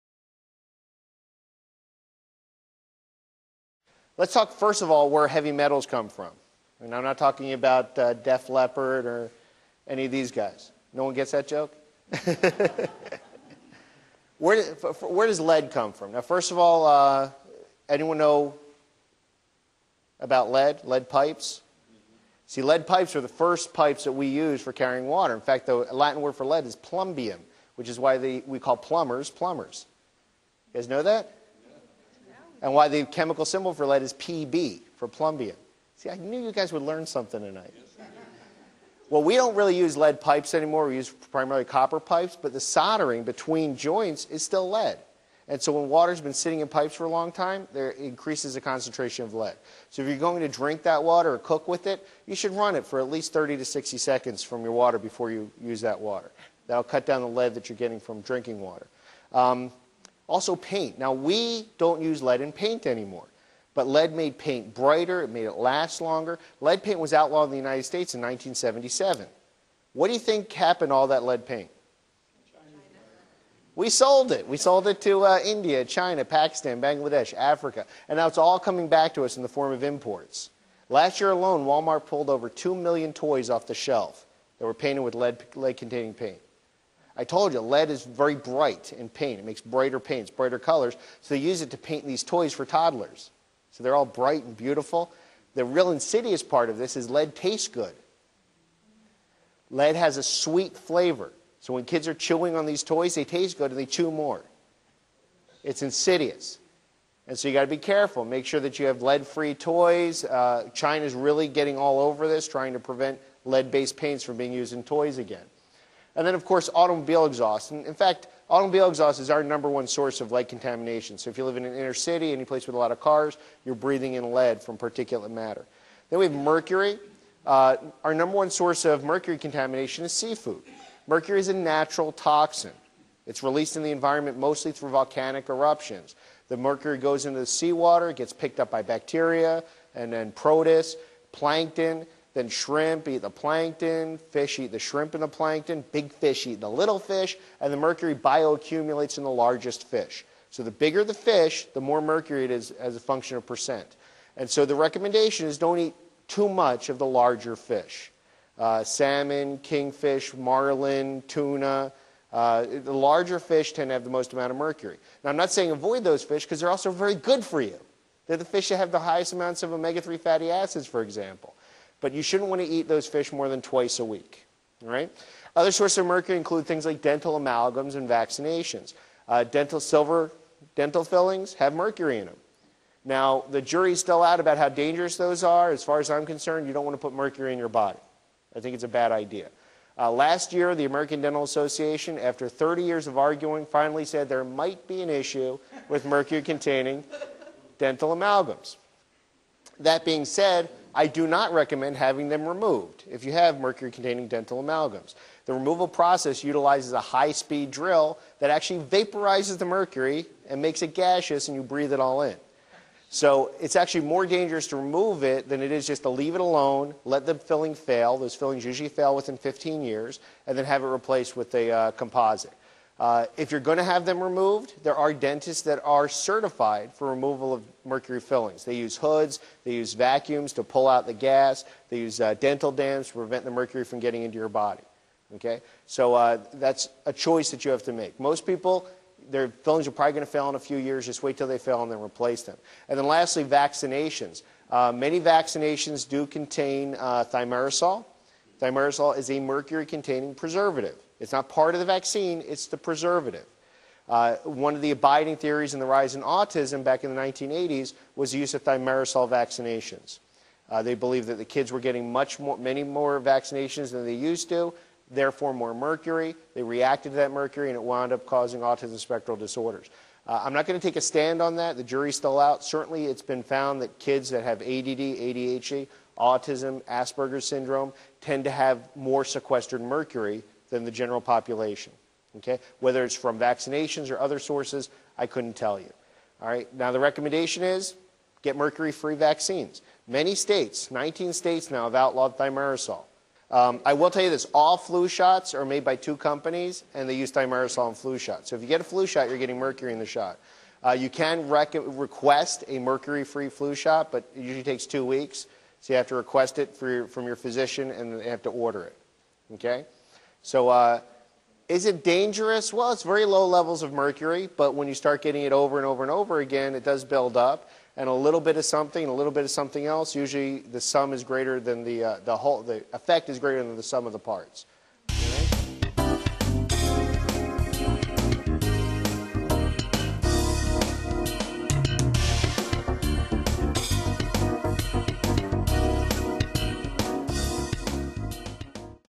Sources of Heavy Metals - Part 1
In part 1, Rik Deitsch, Biochemist talks about lead and mercury.
Even though we don't use lead pipes anymore, lead can still be found in the solder that joins copper pipes. Best to let your water run for a minute, so the water that has been sitting in the pipes can flush out.
Mercury is found in seafood, dental amalgams and vaccines, just to name a few sources.